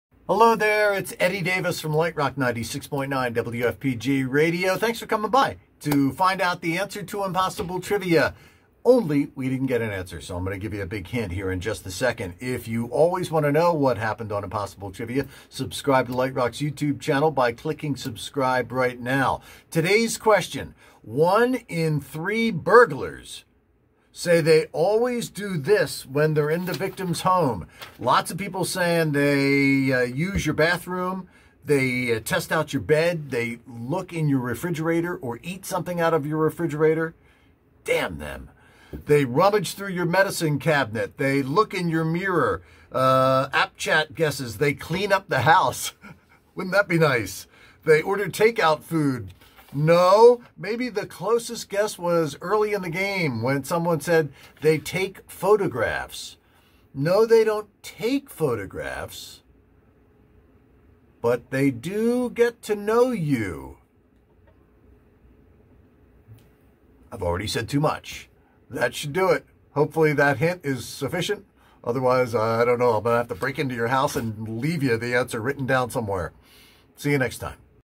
1 in 3 Burglars Always Do This in Victim's Home? IMPOSSIBLE TRIVIA
Today's Impossible Trivia question: One in three burglars say they always do this in a victim's home. What is it?